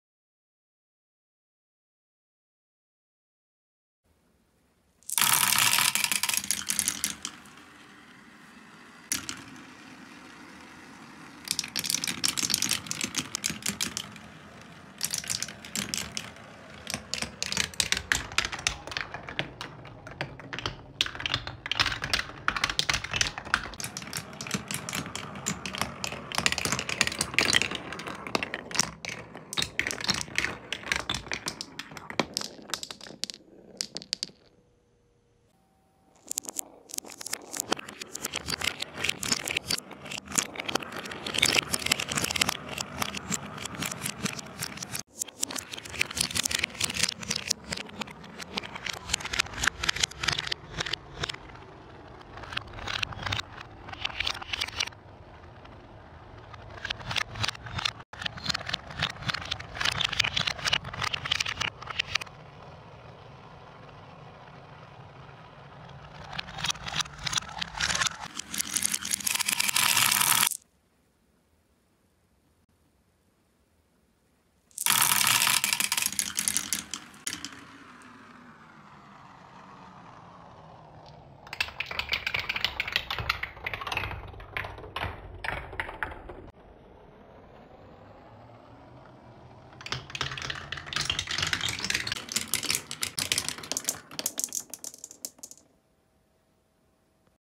【Quadrilla】★マーブルレースぐるぐるスロープ#quadrilla
回転スロープ3個使ってみました。

ピタゴラスイッチもやってるので使ってほしいものがあったらコメント下さい。